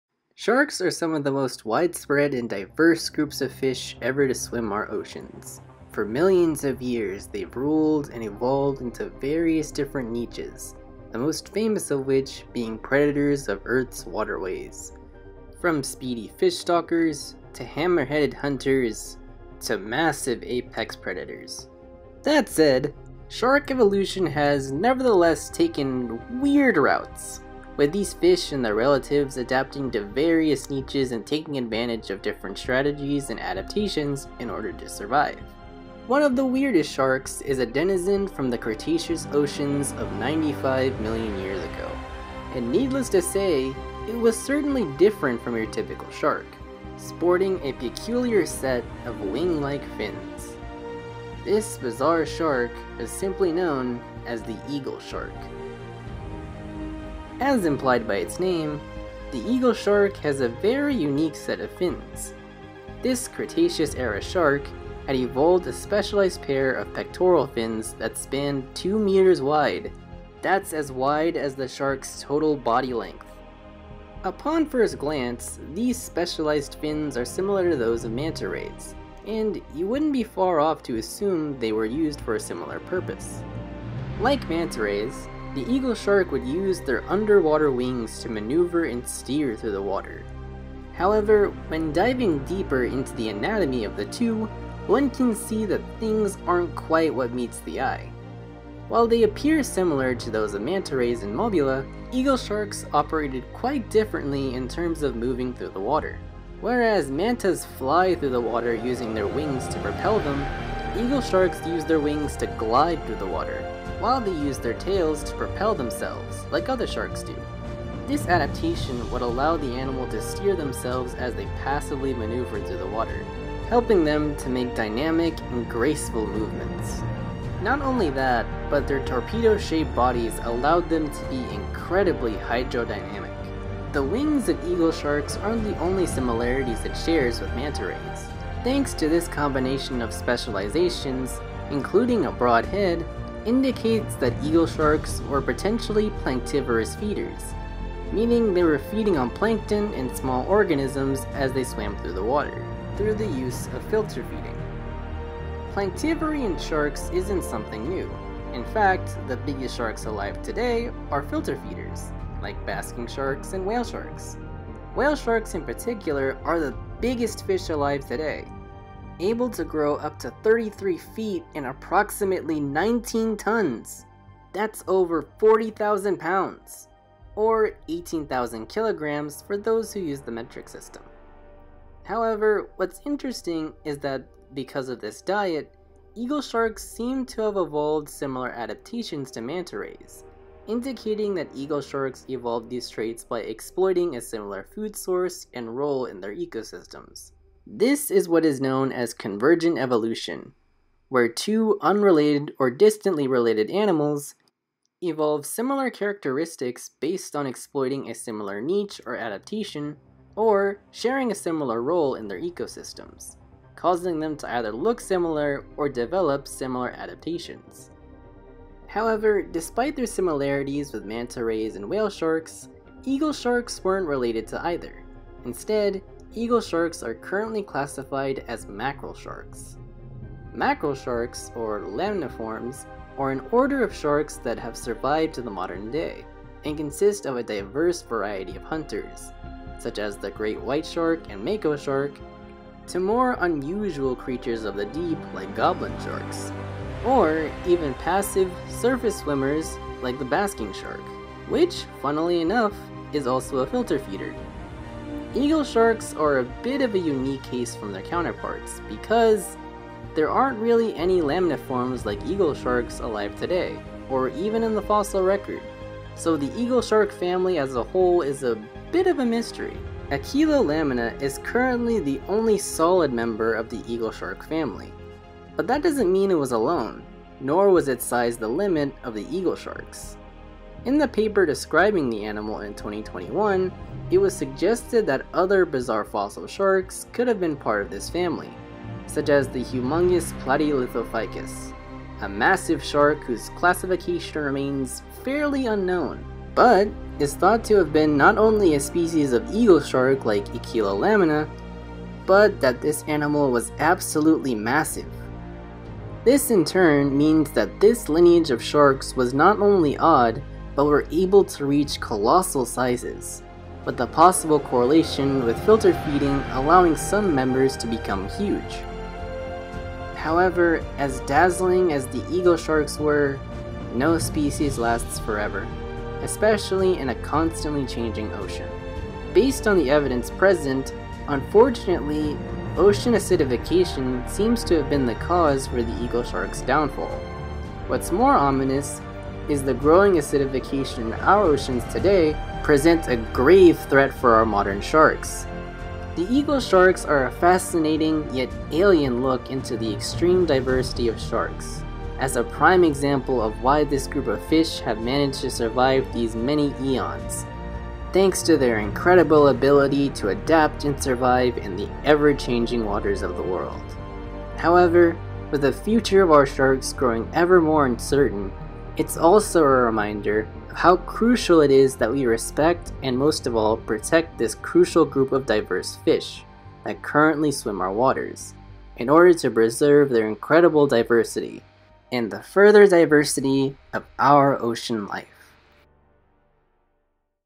Paleo Log: Eagle Sharks
Happy (Late) Shark Week! This time we'll be exploring the diversity of earth's greatest fish with one of the most unique sharks ever to have evolved! The Eagle Shark!